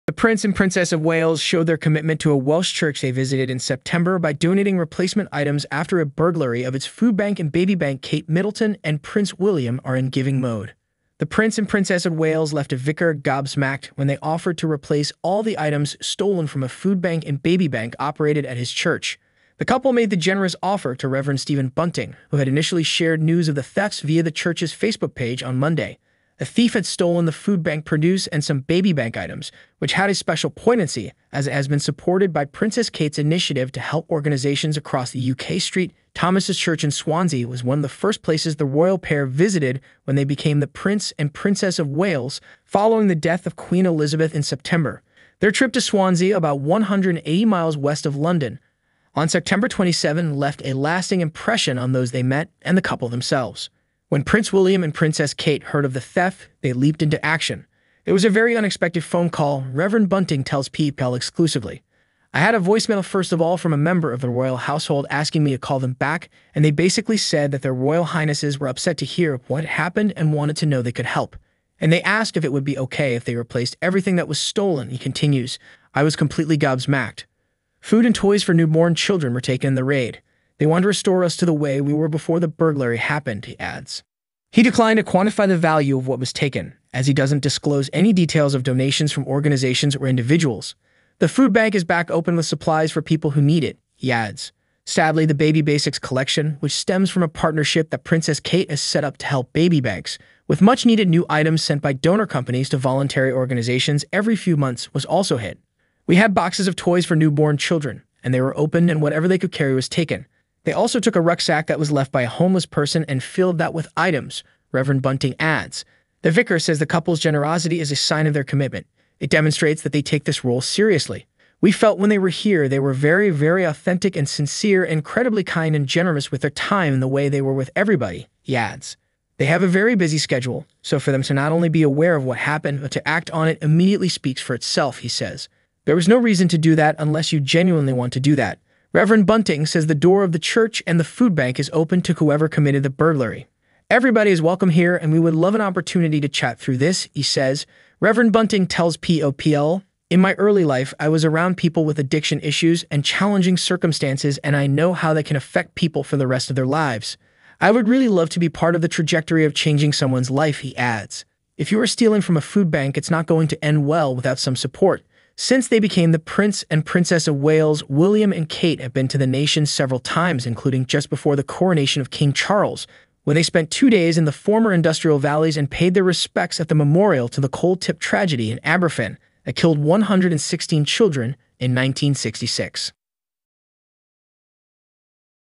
Kate Middleton and Prince William's Generosity Leaves Welsh Vicar 'Gobsmacked' Following Theft
Kate Middleton and Prince William have once again displayed their generosity by offering to replace stolen food from a Welsh food bank. The couple's act of kindness left a local vicar "gobsmacked" as they reached out to help after hearing about the theft. The Duke and Duchess of Cambridge are known for their charitable endeavors and have consistently shown their commitment to supporting those in need. This latest gesture demonstrates their genuine care for the communities they engage with and their willingness to step in when help is needed. Kate and William's generosity continues to inspire and make a positive impact on the lives of others.